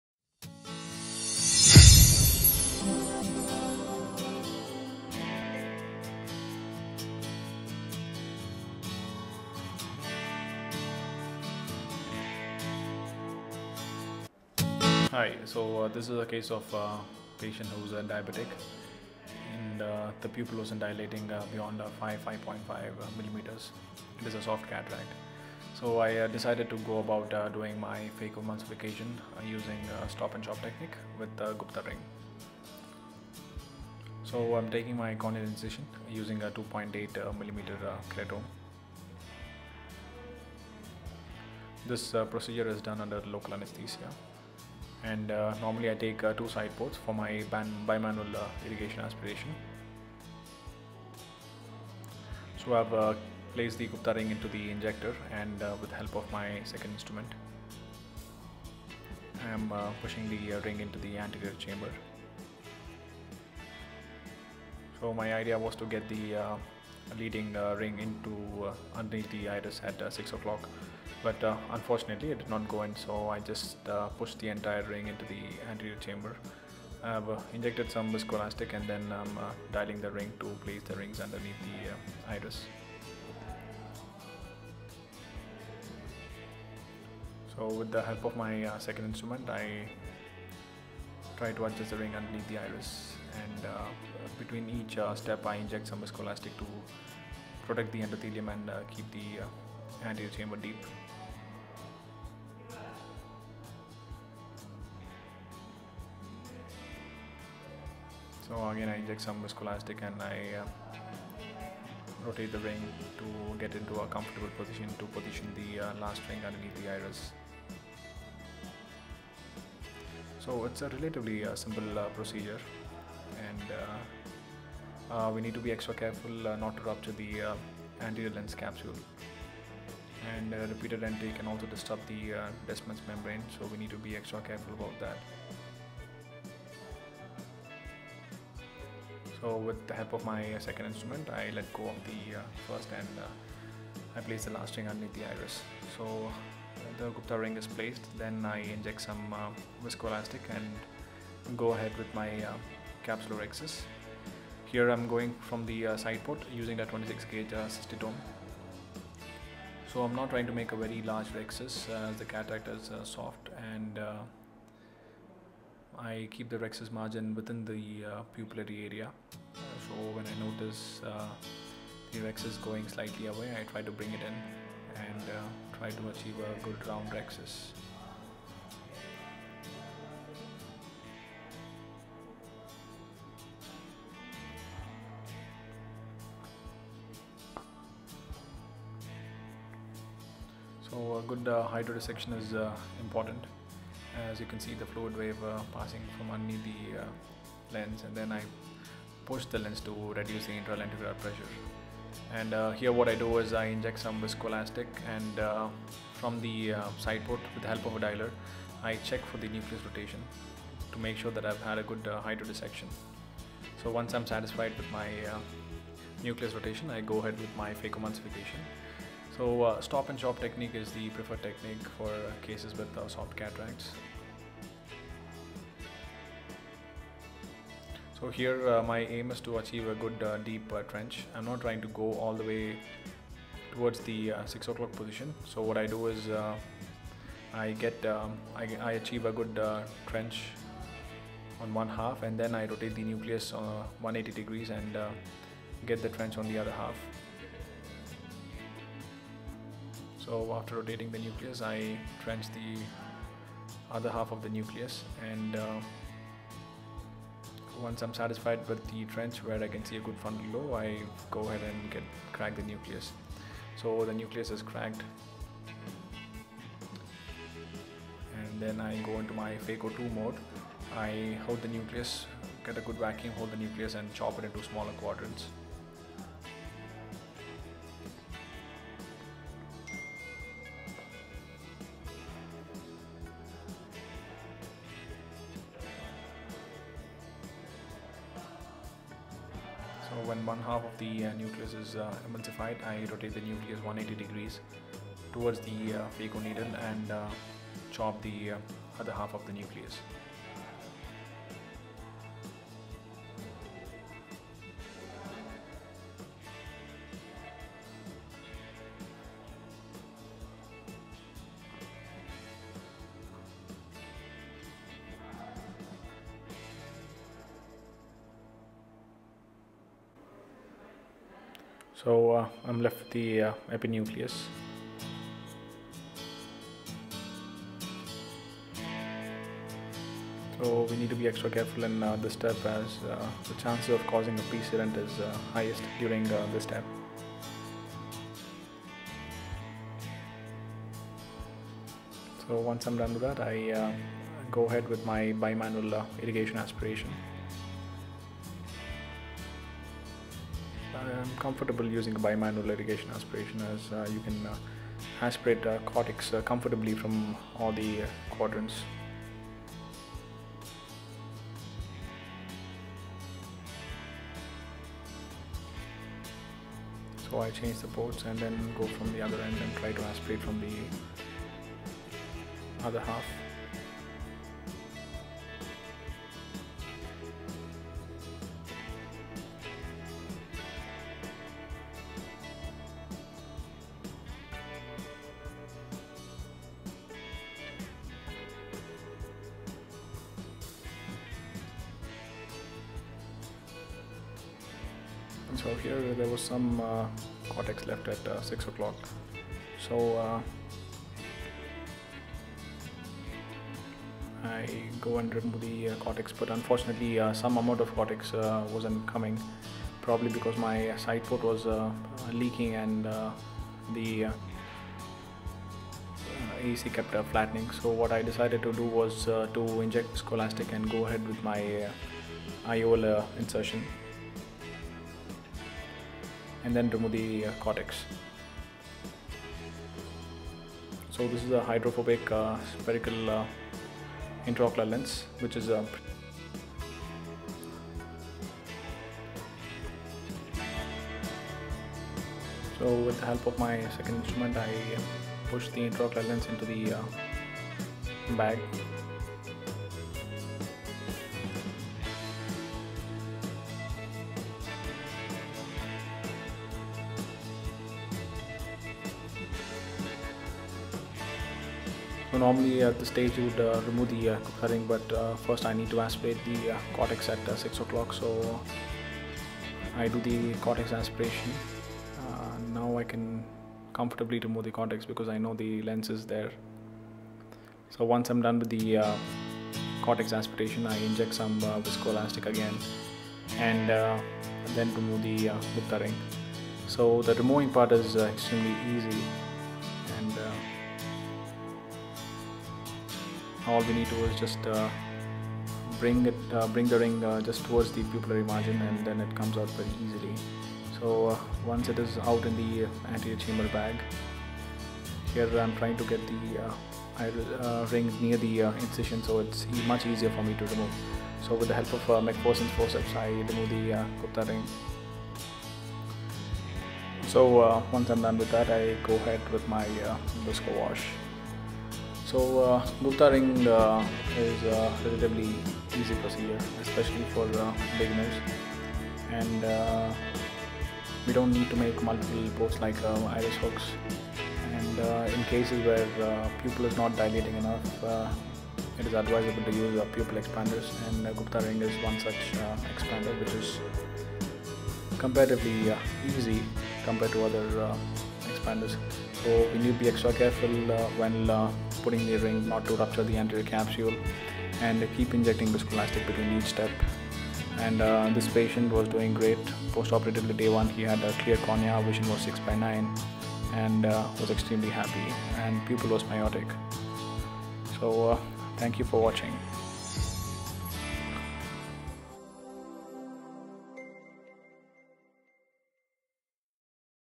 Phaco in small pupil using gupta ring - 4k video
Performing phacoemulsification in small pupils is always challenging . Preoperative planning is essential in such cases using iris hooks , Gupta ring or other devices for better intra and post surgical outcomes  . Care must be taken not to puncture the anterior lens capsule and repeated injection of viscoelastic helps protect the endothelium and keeps the cornea clear on post operative day 1. Gupta ring is a relatively simple procedure , should be performed under local anaesthesia especially for beginners .